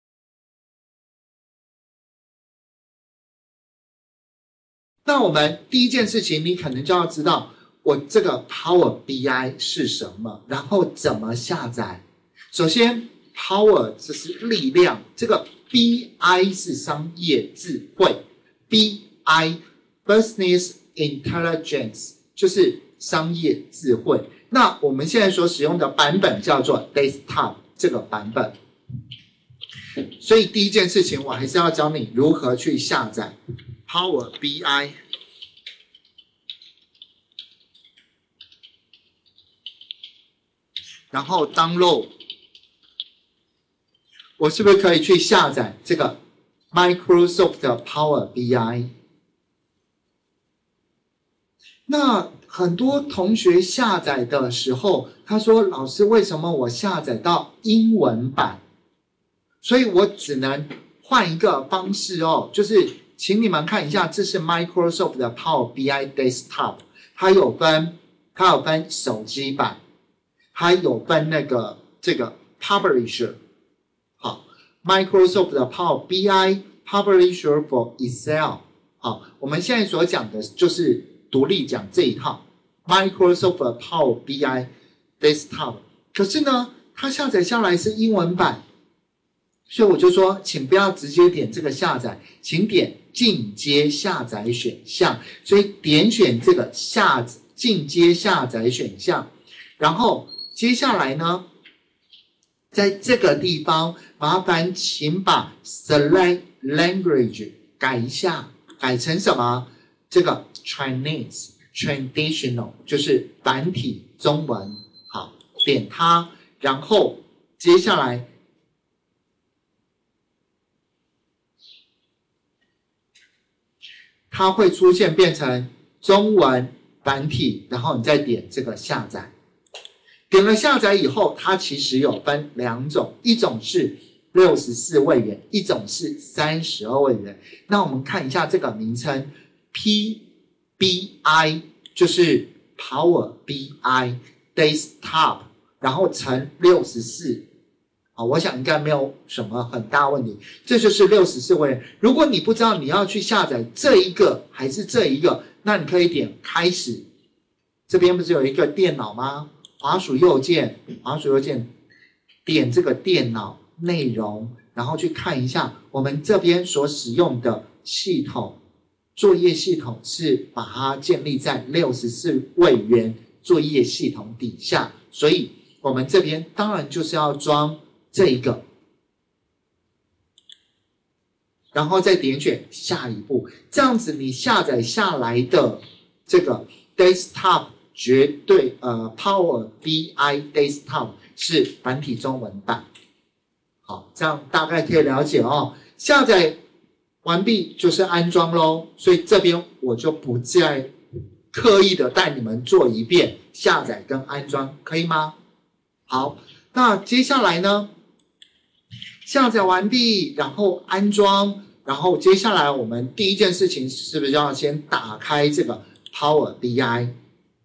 14.Power BI Desktop繁體中文版下載(Google Analytics網站分析)
14.Power BI Desktop繁體中文版下載
Google Analytics 是一個Google開發的免費線上工具，主要功能是進行網站資料分析，並以視覺化分析呈現結果。會使用GA工具並不代表會網站分析，因為每一個人都可以輕易申請好GA的使用權，都可以輕易看到GA的報表，視覺化分析是很重要的學習重點。
如何進行網站分析、分析什麼?這才是本門課的重點。本課程以自己的網站被瀏覽次數為資料來源，使用Power BI軟體做資料分析，並找出行為模式，找出瀏覽者地理位置、瀏覽行為、喜歡哪些網頁；真正達到自己的網站、自已分析的目的。